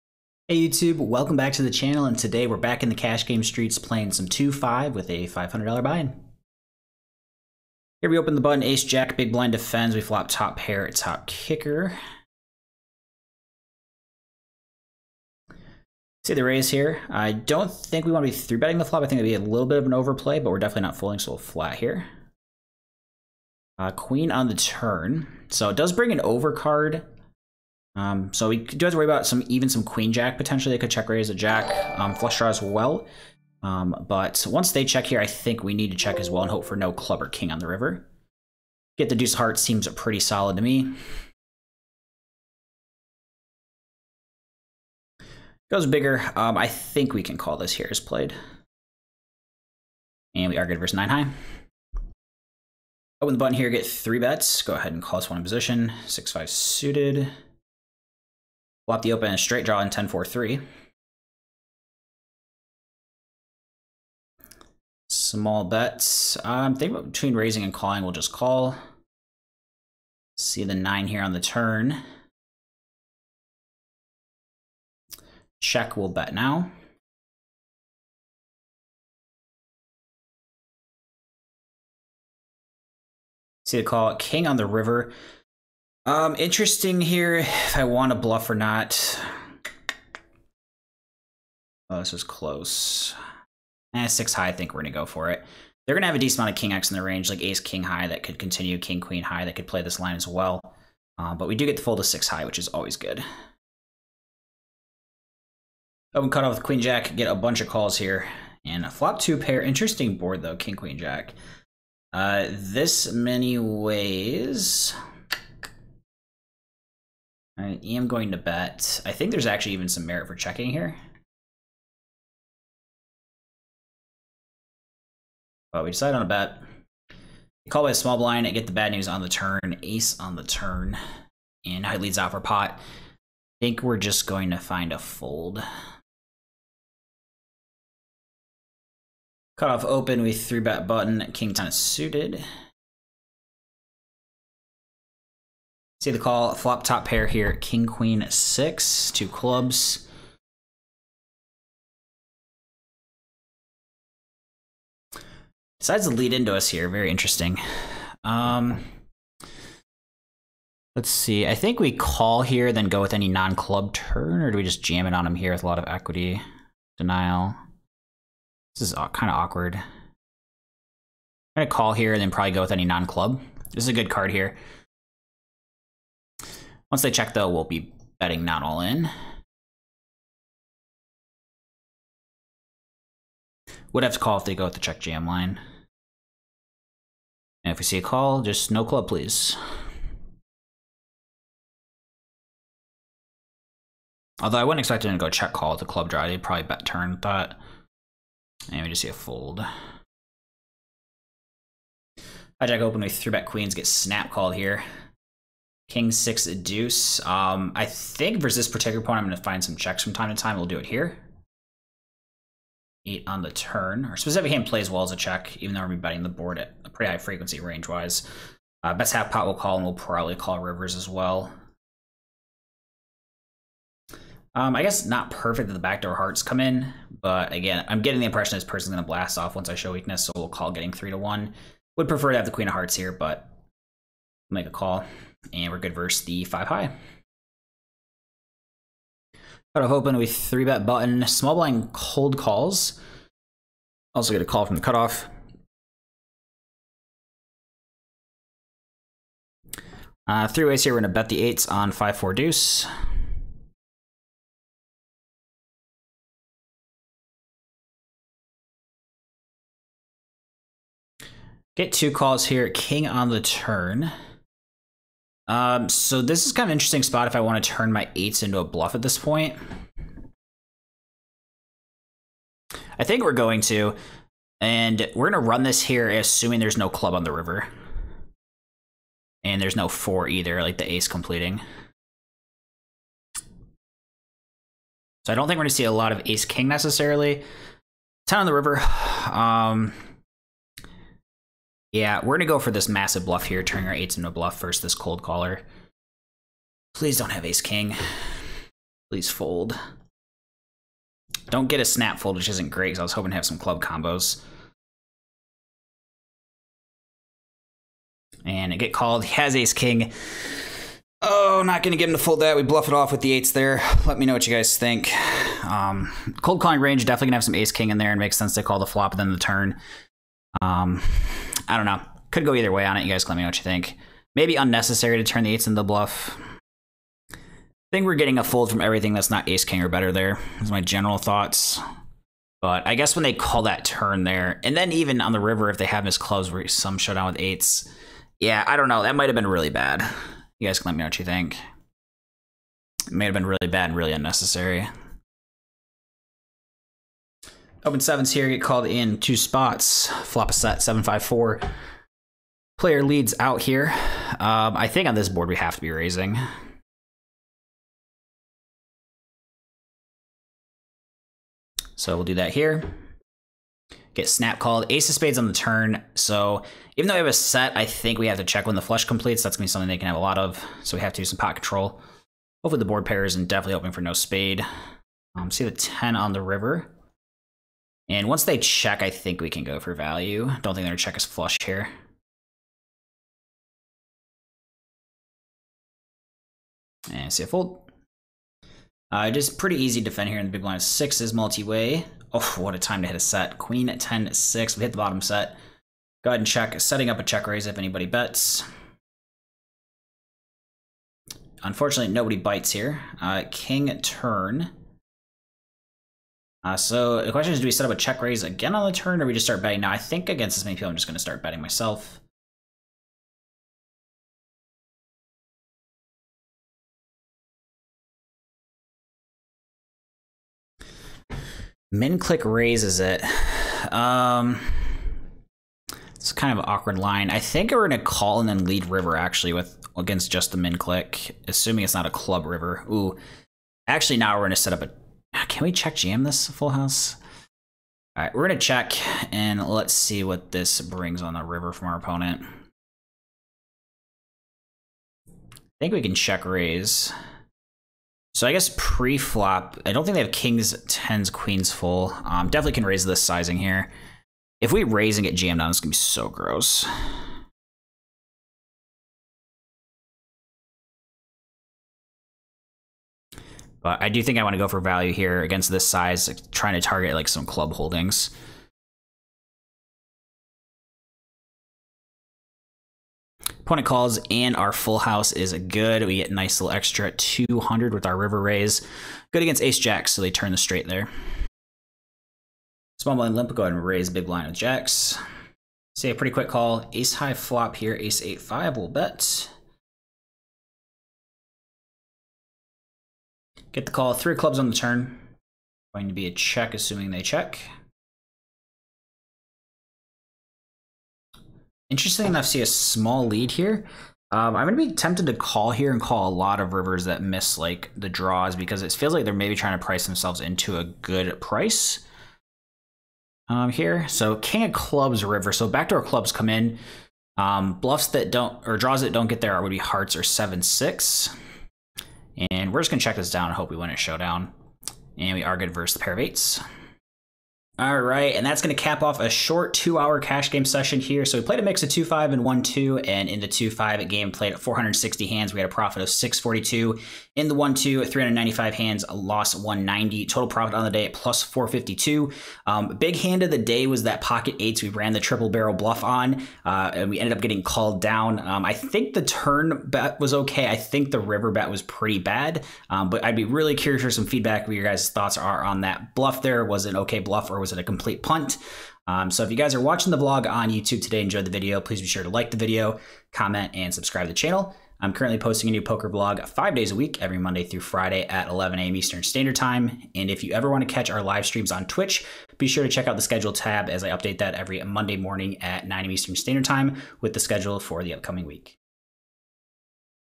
Bluffing ALL-IN For $500 With Eights | Poker Vlog #254
Poker Vlog 254: I find myself on the river with a pair but I decide to turn it into a bluff by going all-in. Can we get the fold?